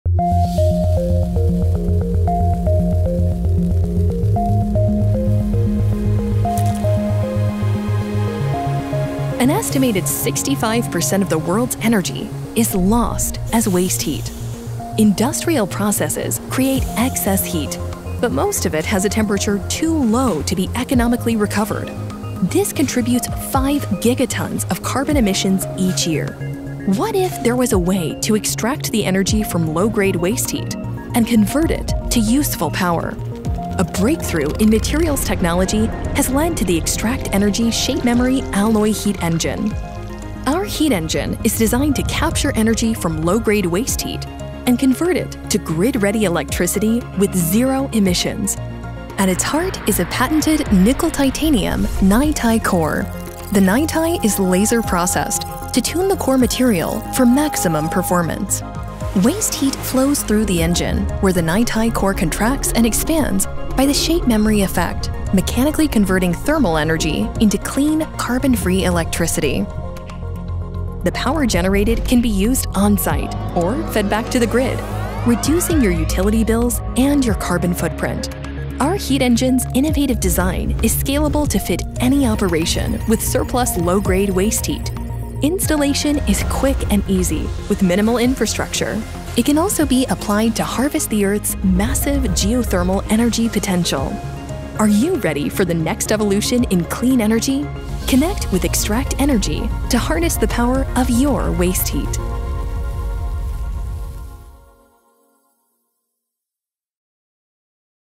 Extract Energy's Waste Heat Shape Memory Alloy Cleantech Heat Engine
An estimated 65% of the world's energy is lost as waste heat. This video shows what our innovative Extract Energy Heat Engine can do to capture and convert that waste heat - turning it into clean, grid-quality, carbon-free electricity.

to learn more, and follow Extract Energy on social